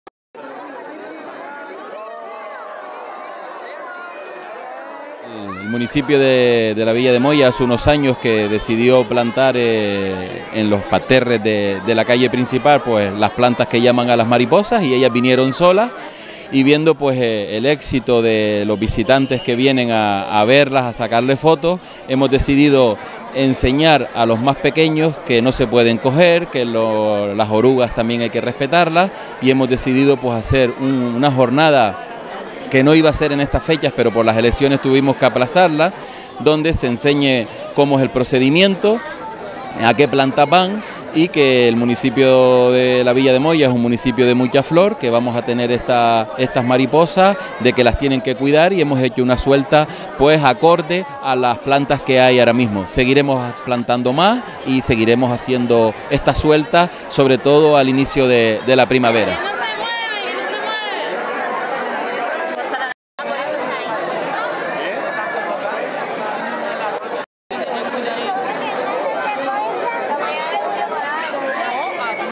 Las mariposas monarcas llenan de color la villa grancanaria de Moya
‘Moya en Flor’ se desarrolló en la Villa de Moya, en Gran Canaria. Una actividad innovadora que atrajo la atención de numerosos vecinos y visitantes que no quieren perder la ocasión de vivir de cerca la suelta de mariposas monarcas. La tarde comenzó a las 17.00 horas con los talleres, unos talleres dedicados a toda la familia con actividades en la que descubrir los secretos de la mariposa monarca y llevarse algún que otro recuerdo, un taller dulce, en el que pudieron realizar y degustar una rica mariposa monarca y, finalmente, un taller divulgativo en el que conocieron todos los pormenores de la vida de las mariposas monarcas y los flujos migratorios de las mismas.

“Ha sido una actividad pionera que está causando sensación entre todos los visitantes. Estamos acostumbrados a ver mariposas monarcas en nuestras calles pero ver y vivir la suelta de la tarde de hoy ha sido muy bonito y ha generado una gran expectación”, destaca el alcalde Raúl Afonso. Unas palabras a las que se suma la concejala del área, Belén Rivero, “estamos encantados con la respuesta que ha tenido la iniciativa. Es un proyecto que estamos desarrollando pero que va más allá de la tarde de hoy con la instalación de las macetas con asclepias para que perduren en el municipio”, resaltó.

La Villa de Moya vivió un espectáculo único y pionero en Gran Canaria con una suelta de mariposas monarcas que cautivó a pequeños y mayores, que no quisieron faltar a la cita.